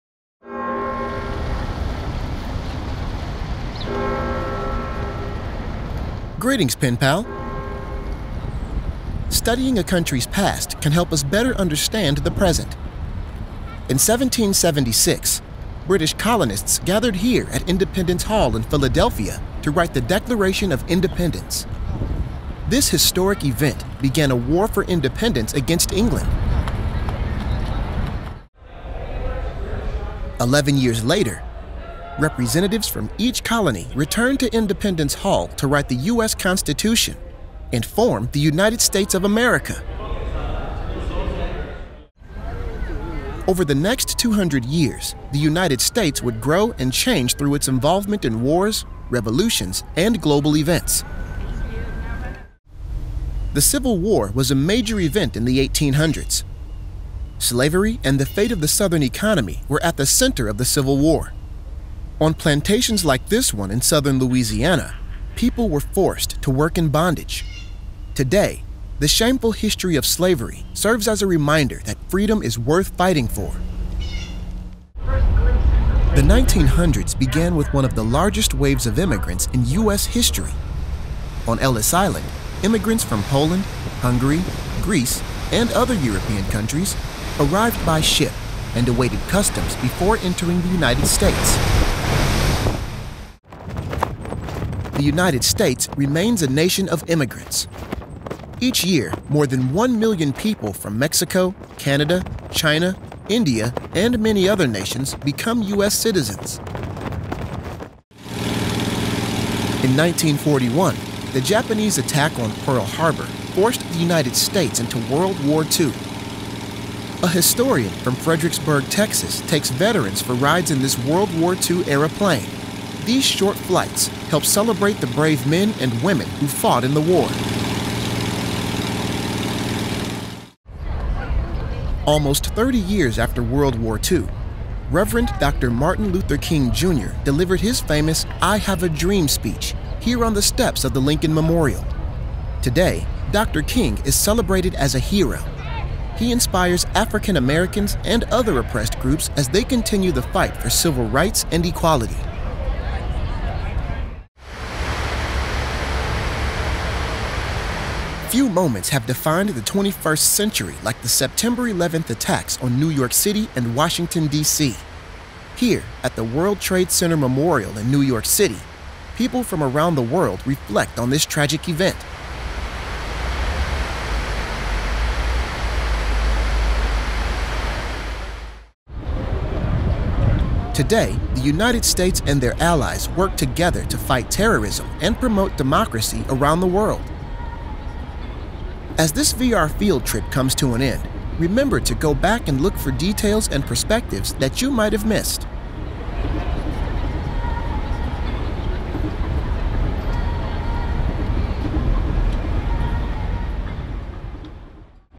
VR Field Trip: USA - History
Students and teachers explore the United States of America in a new and exciting way through the PenPal Schools VR Field Trip: USA!